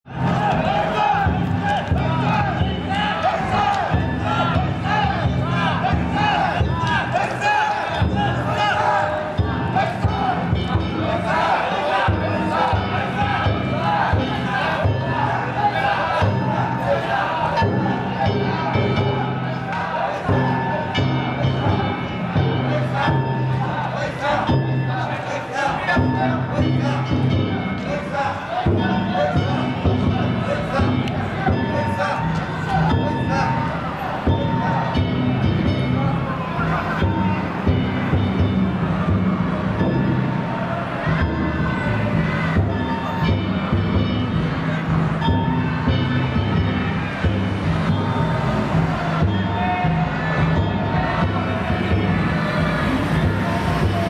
平成29年大東市市制60周年記念大東地車大集結３
寺川地車が会場へ向かう様子です。